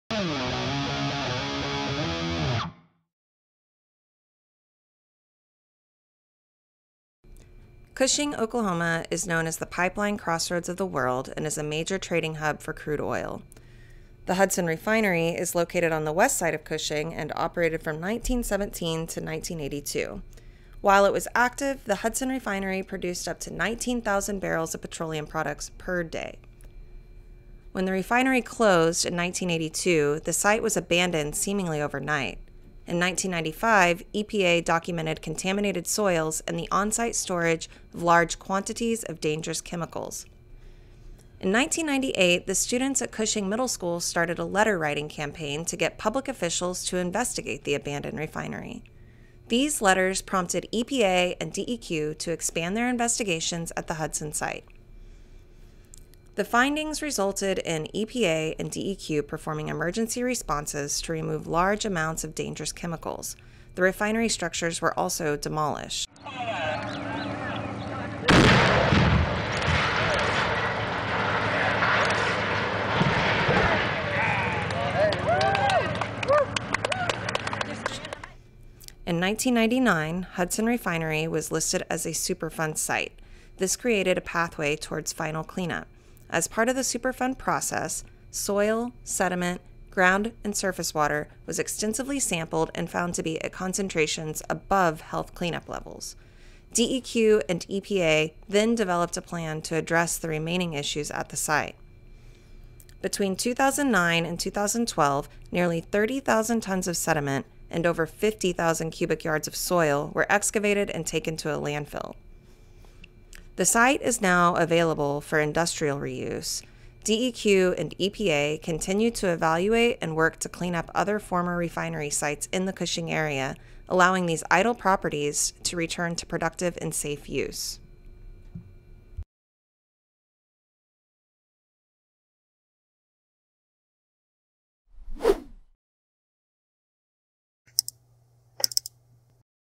Hudson Refinery
A look back at the former Hudson Refinery and how DEQ made the land reusable!